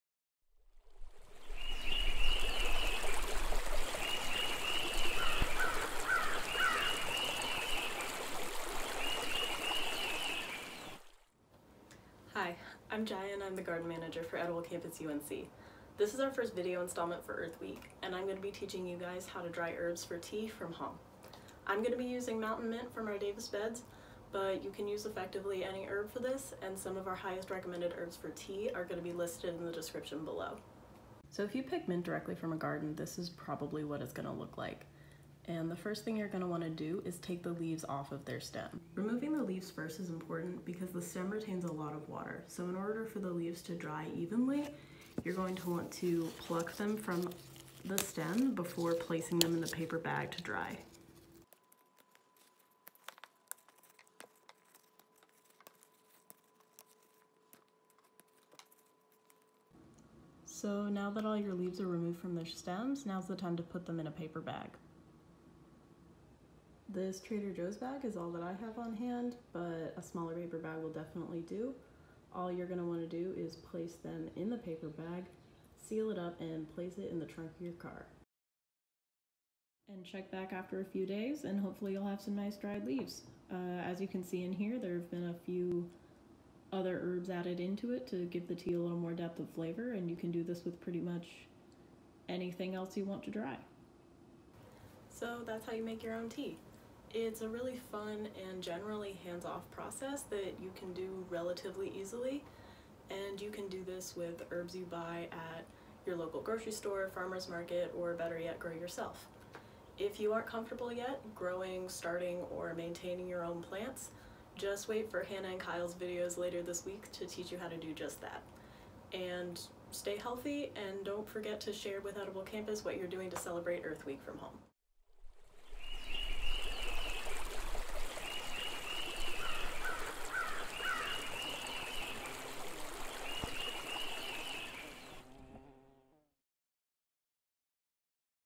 EarthWeek Day 1: Herbal Tea Making
Beginners lesson on some of your favorite garden herbs and how to make herbal tea at home.

Starring Jaya Kuic '21, Edible Campus Garden Manager
Video Editing by CriShaun Hardy '20, Edible Campus Communications Co-Manager

Resources:
Great article on making herbal tea by Mountain Rose Herbs:

Thyme, Mint, Chamomile, Hibiscus, Lavender, Lemon verbena, Lemon balm, Ginger, Stevia, Rosemary, Fennel, Sage, Basil

Also check out our UNC friends at Herban Gardening Collective -- one of our herbal tea inspirations!